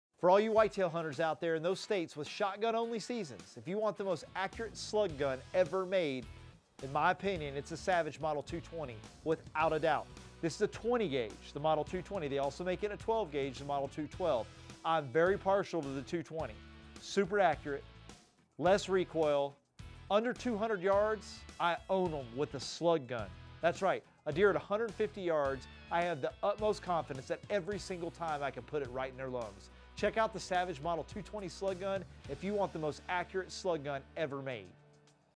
Savage Model 220 Slug gun
Mike Stroff from Savage Outdoors TV explains why the Model 220 is his first option when hunting whitetails in the shotgun states.